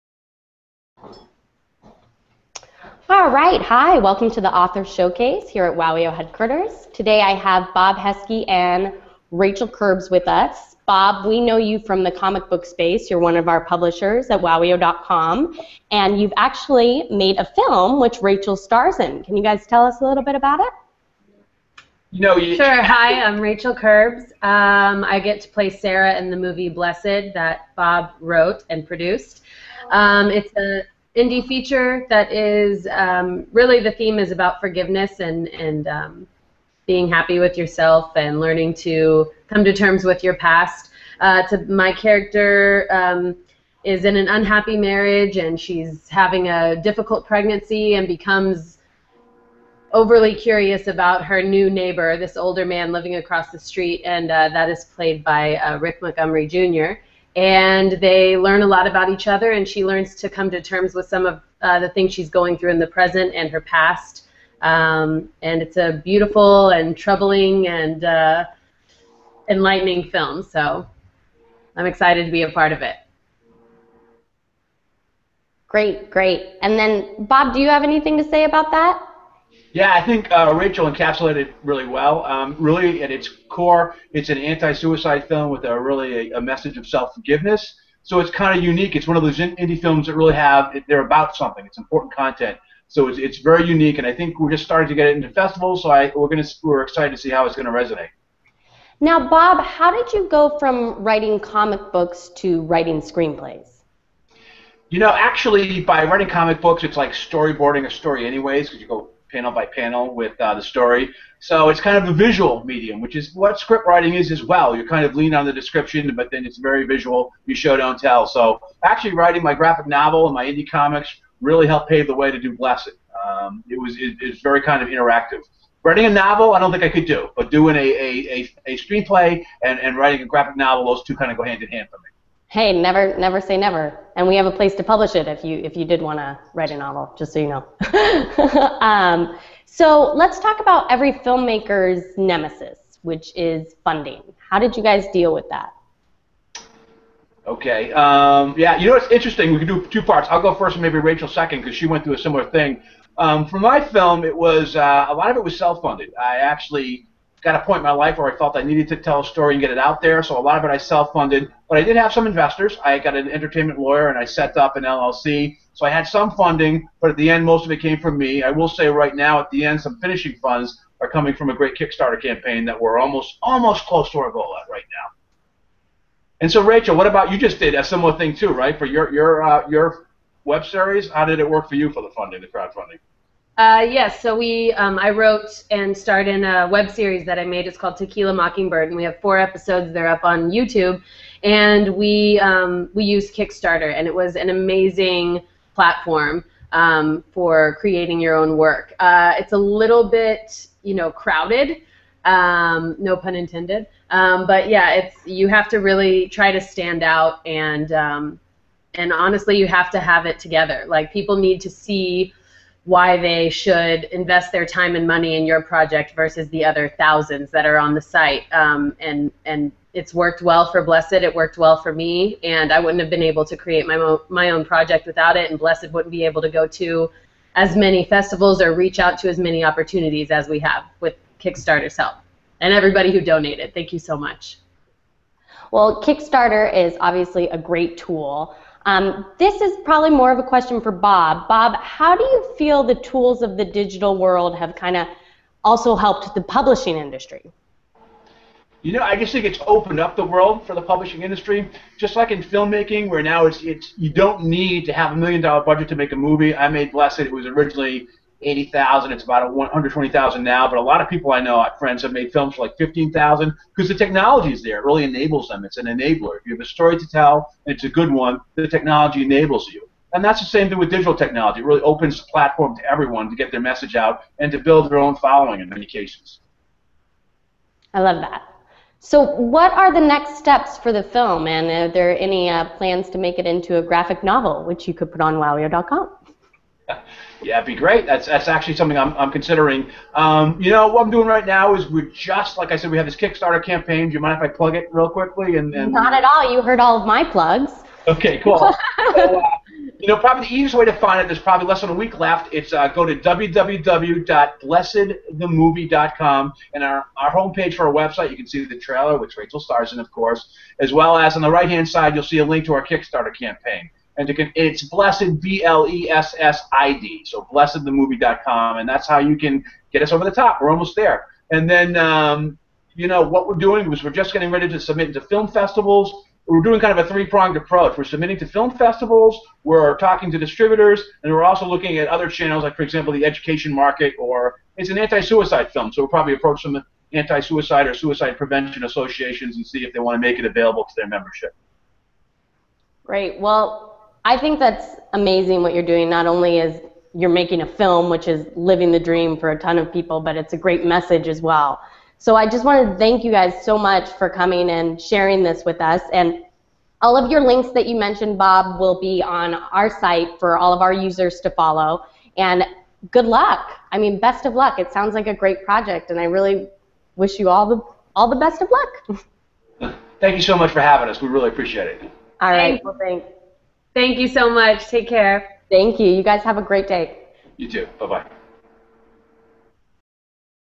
Author Showcase Bob Heske Blessid
PopGalaxy's Author Showcase presents an interview with Bob Heske, Rachel Kerbs, and your host, Emily Cleary.

Writer/Producer Bob Heske, author of Cold Blooded Chillers (available on Wowio.com), along with actress Rachel Kerbs tells us about their new film Blessid, a suspenseful indie drama about a pregnant woman with a cursed past who forms a bond with a man who is immortal. At it's core Blessid is an anti-suicide film whose ultimate message is about self forgiveness

CREDITS:
Executive Producer: Brian Altounian
Producer: Jacob Morris
Host: Emily Cleary